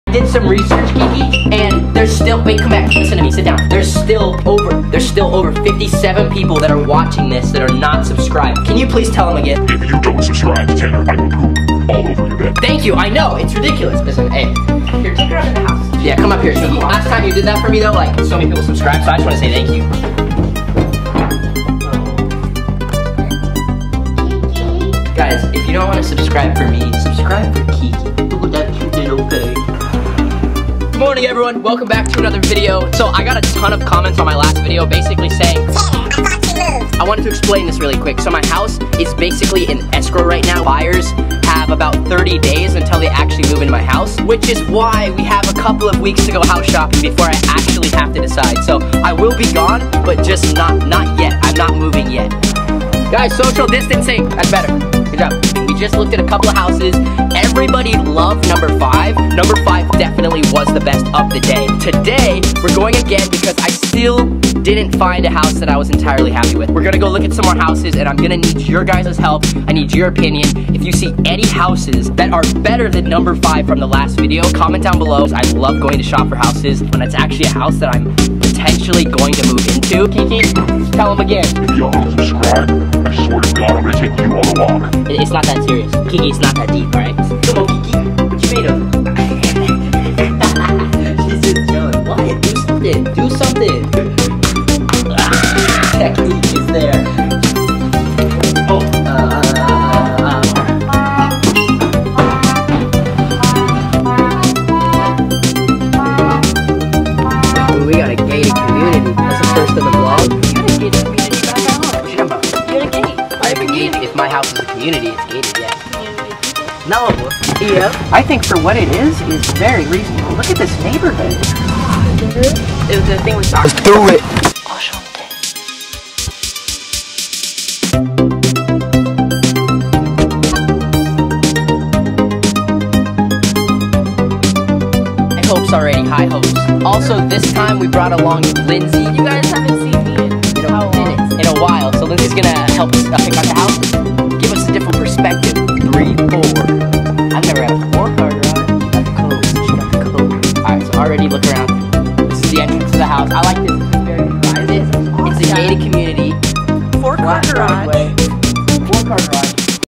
Custom Editor Remix 14 ( Tanner Fox Remix ) THE TANNER FOX RAPP 💿
Please enjoy the video of Tanner Fox rapping with the music I added to the remix and I cut most of the video and that’s why it is only 2 minutes long and also I mute his music tracks to make it sound better, accept one part of the remix still has some of his track in it, but that’s ok, I hope you guys enjoy the Tanner Fox rapping with the music and please make sure you leave a like and subscribe and comment down below.